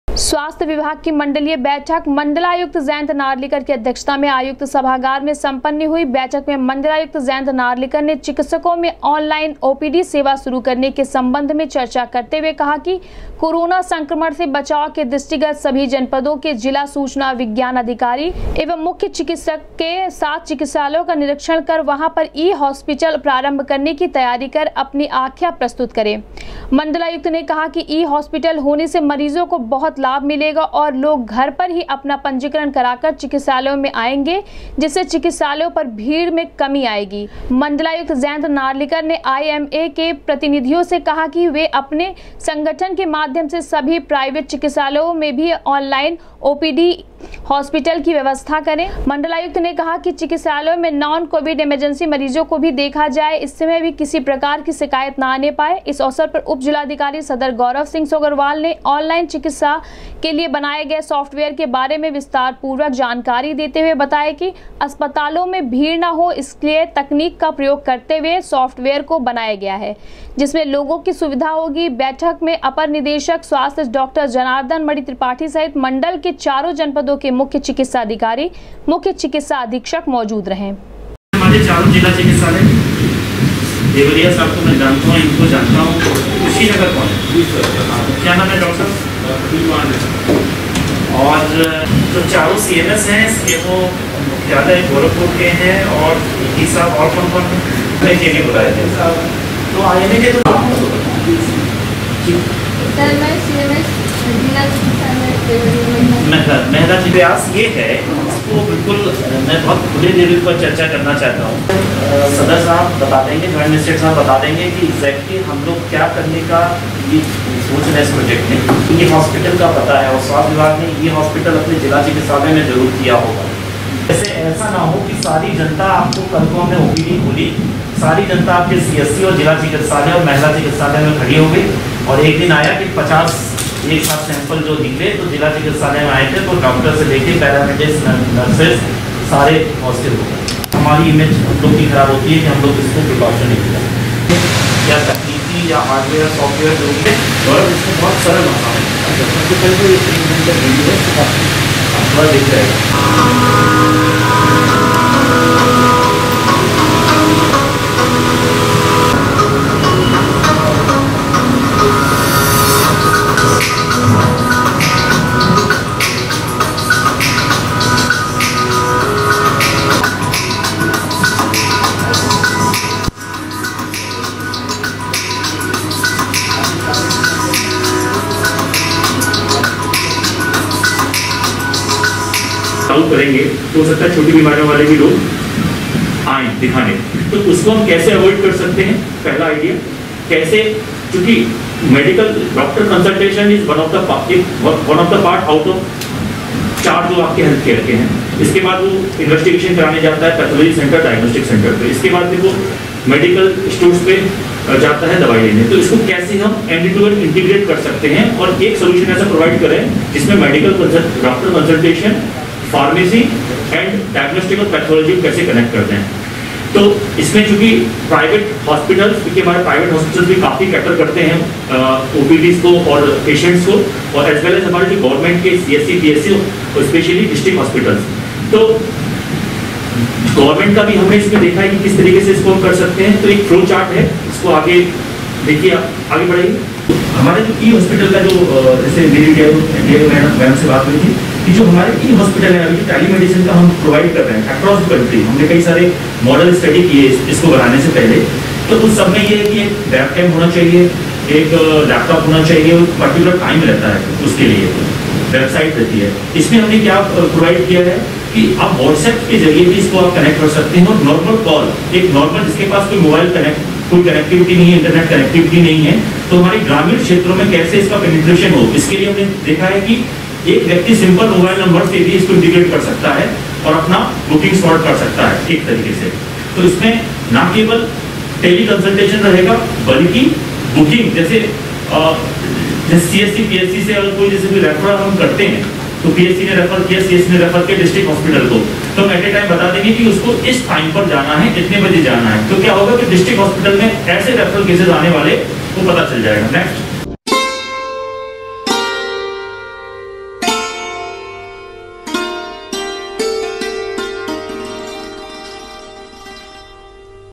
मण्डलायुक्त जयन्त नार्लिकर की अध्यक्षता में सम्पन्न हुई स्वास्थ्य विभाग की मंडलीय बैठक
मण्डलायुक्त जयन्त नार्लिकर की अध्यक्षता में सम्पन्न हुई स्वास्थ्य विभाग की मंडलीय बैठक,सरकारी व गैर सरकारी हॉस्पिटल में आन लाइन ओपीडी चलाने पर हुई चर्चा, मण्डलायुक्त ने कहा,ई-हॉस्पिटल से होगा सोशल डिस्टेंसिंग का पालन